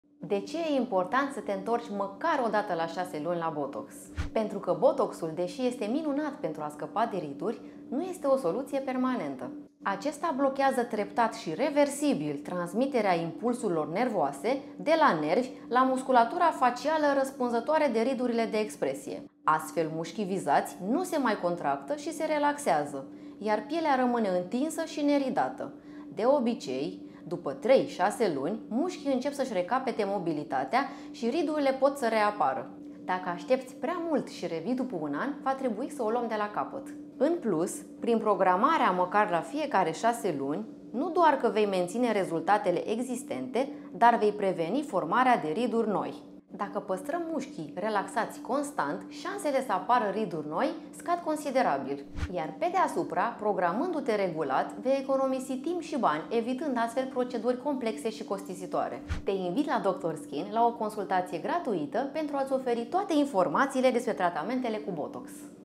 A doua ședință după 6 luni?👄Injectări cu Toxină Botulinică💉Doctor SKiN💚
De ce este important să te întorci după 6 luni pentru Botox?

❗Toxina botulinică, deși este o soluție ideală pentru a scăpa de riduri, nu este o soluție permanentă.
❗Botoxul blochează treptat și ireversibil transmiterea impulsurilor nervoase, de la nervi, la musculatura facială responsabilă pentru ridurile de expresie.
❗De obicei, după aproximativ 3-6 luni, mușchii încep să își recapete mobilitatea și ridurile pot să reapară.
❗Dacă amâni prea mult a doua ședință de injectare cu Botox, medicul specialist este nevoit să înceapă procedura de tratament de la capăt.
❗Dacă te prezinți după 6 luni pentru a doua ședință, vei preveni de asemenea și apariția unor riduri noi.